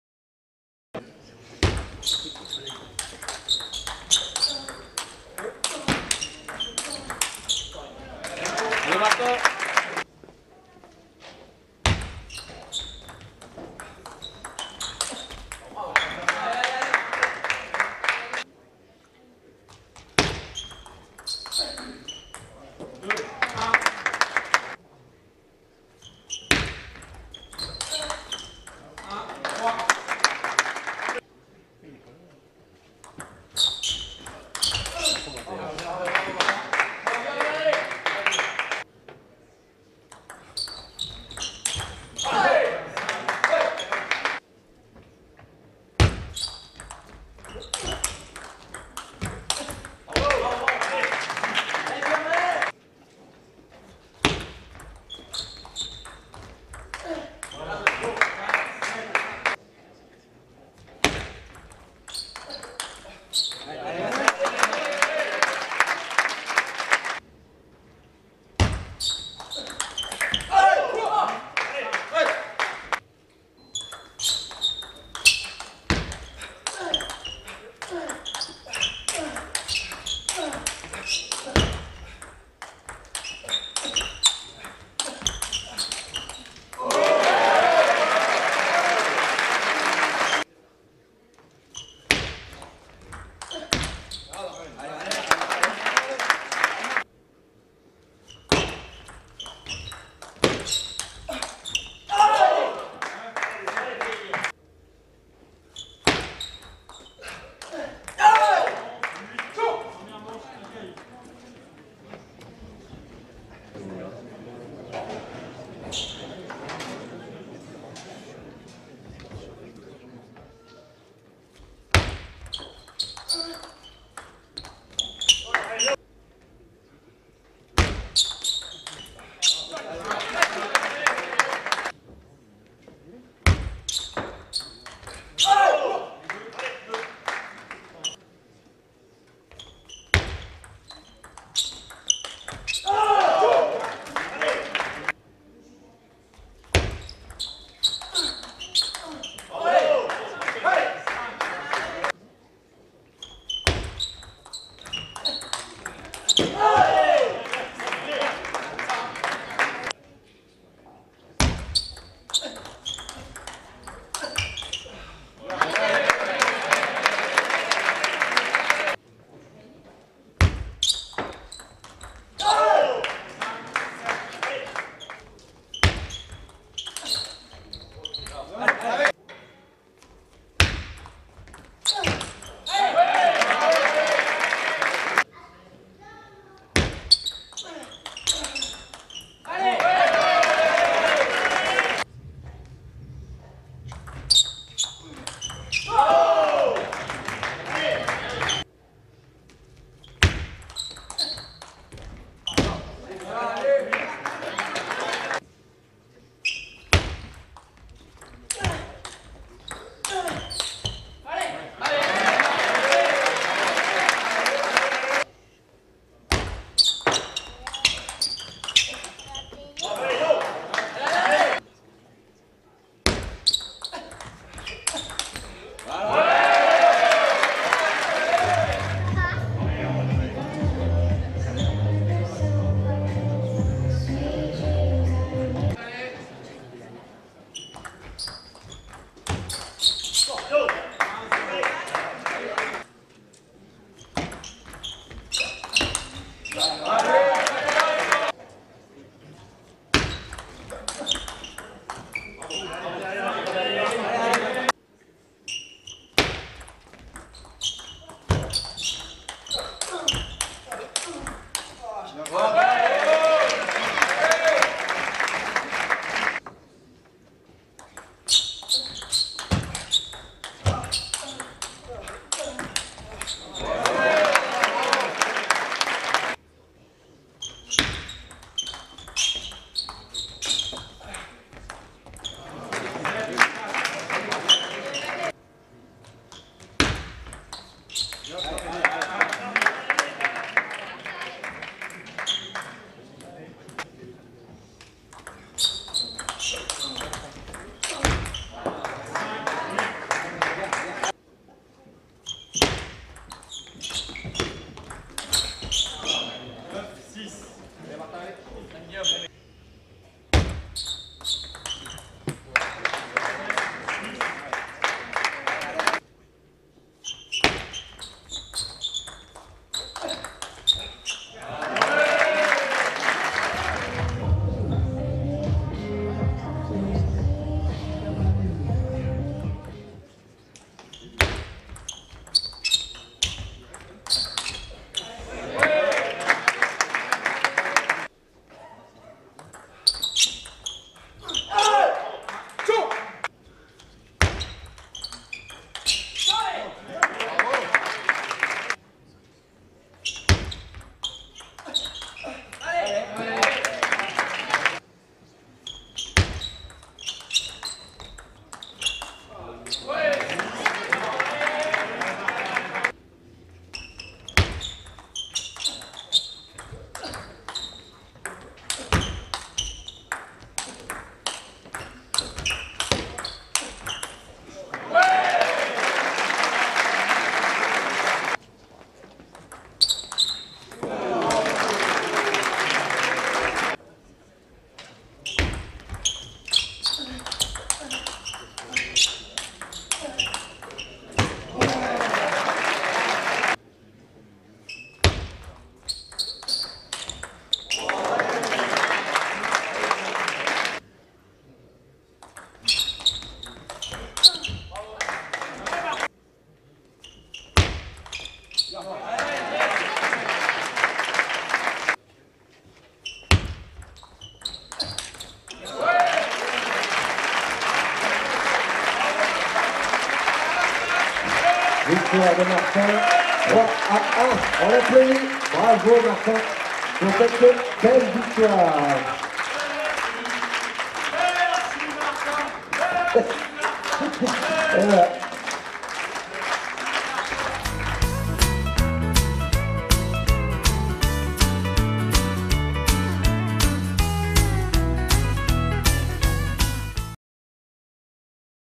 Martin ALLEGRO - Guillaume ALCAYDE (2022)
extrait du match championnat de France Pro B Loire Nord tt / Istres tt à Roanne le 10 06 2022